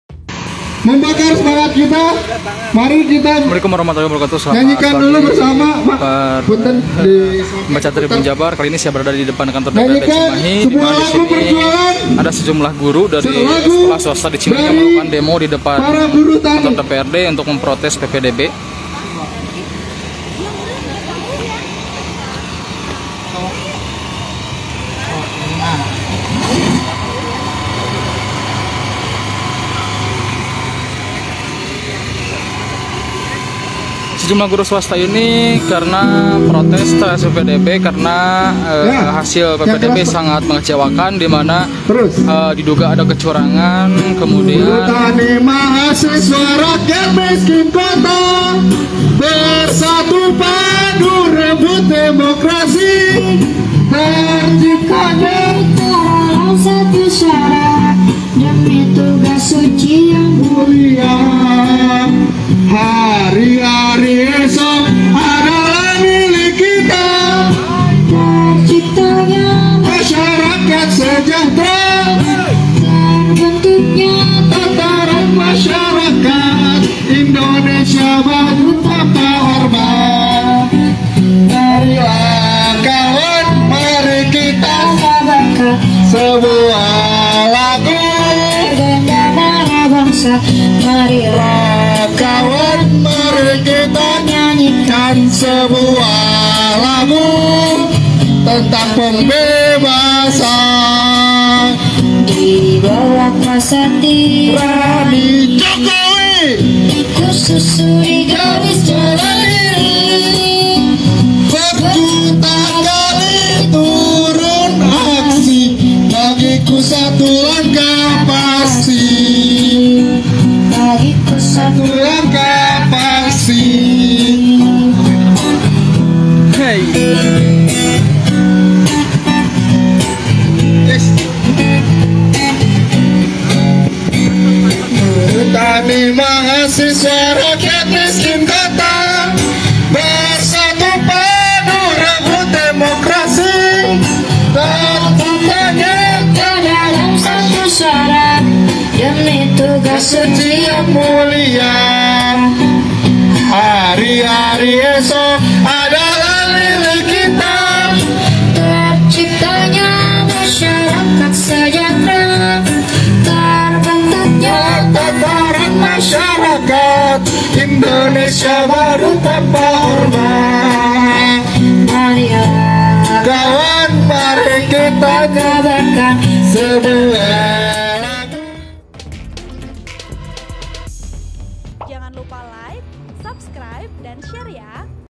Sejumlah guru sekolah swasta di Cimahi demo protes PPDB
Laporan Hilman |Video Editor: Uben